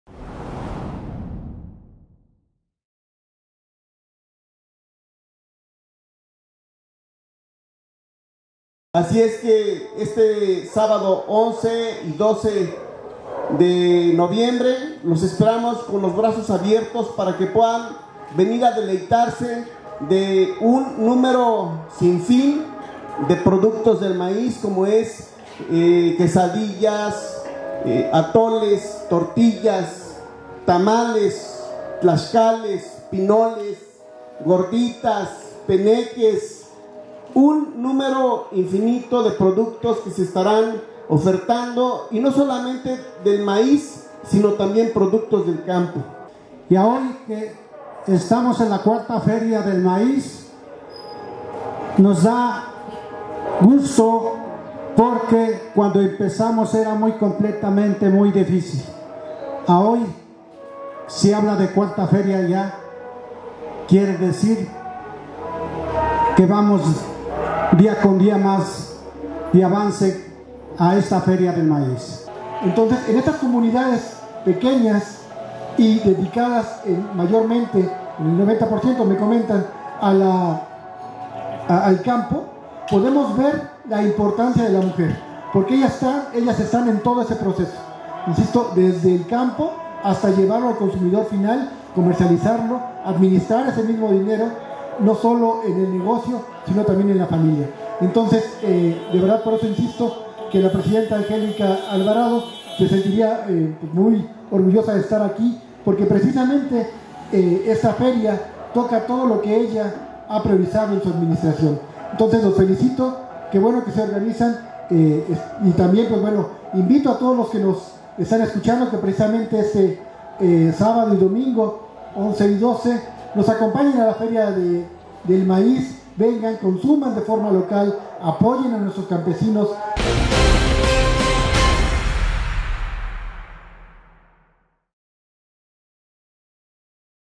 Visita la 4ta Feria del Maíz 2023 en HUEJOTZINGO, PUEBLA.
FeriasTemáticas | Autoridades del municipio de Huejotzingo y organizadores, invitan a la "4ta Feria del Maíz 2023" a realizarse en San Miguel Tianguizolco este 11 y 12 de Noviembre.